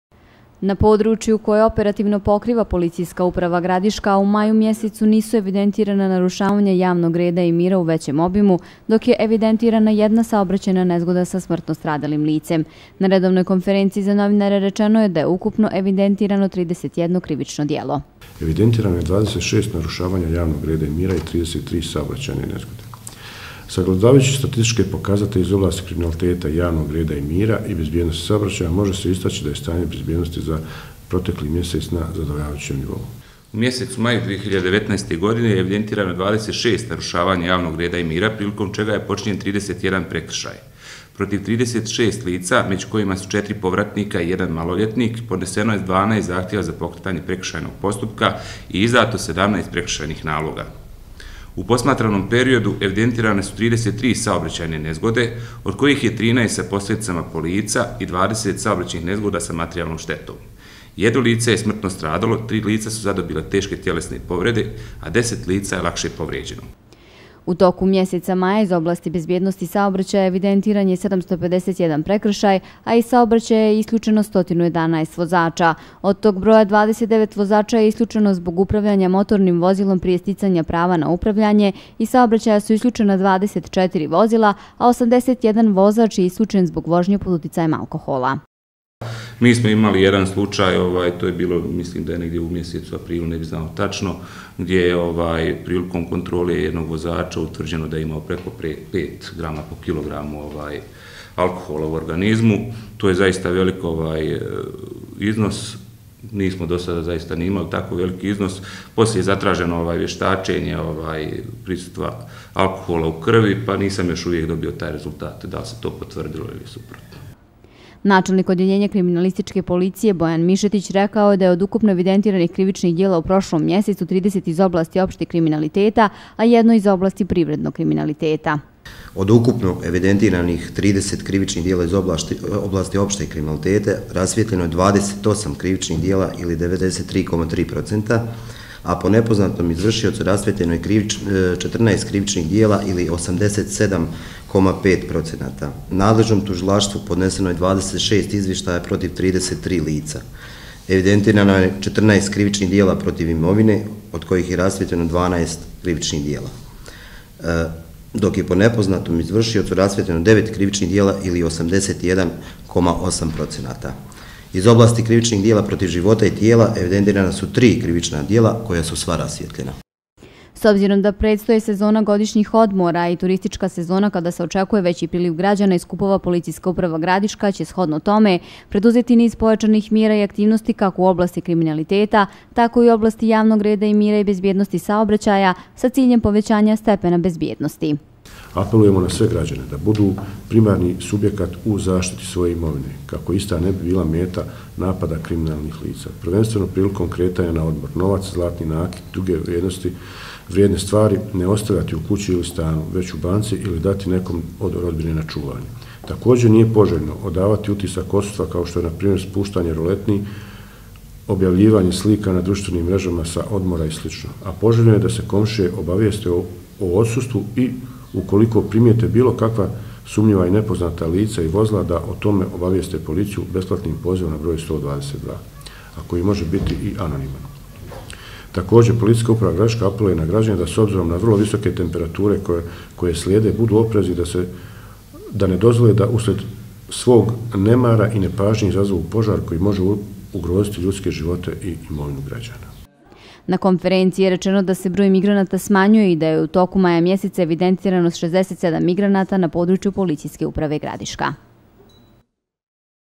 U maju evidentirano 31 krivično djelo
Na području koje operativno pokriva Policijska uprava Gradiška u maju mjesecu nisu evidentirana narušavanja javnog reda i mira u većem obimu, dok je evidentirana jedna saobraćajna nezgoda sa smrtno stradalim licem. Na redovnoj konferenciji za novinare rečeno je da je ukupno evidentirano 31 krivično djelo.